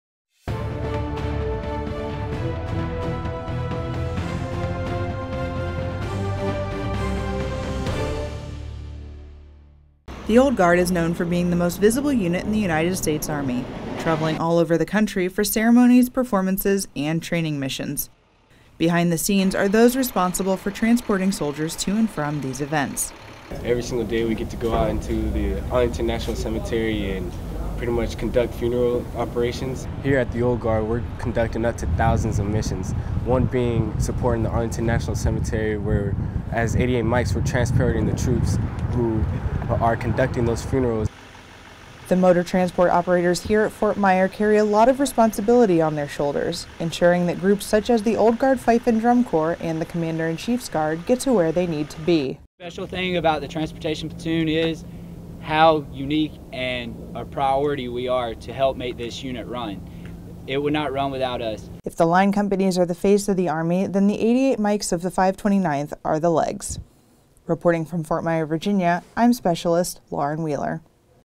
88M Transportation Feature
The 88M's of The Old Guard are responsible for not only transporting soldiers around the cemetery for funeral missions but across the country for parades, ceremonies and community outreach events. They work tirelessly behind the scenes to ensure the companies get to where they need to be in a safe and timely manner.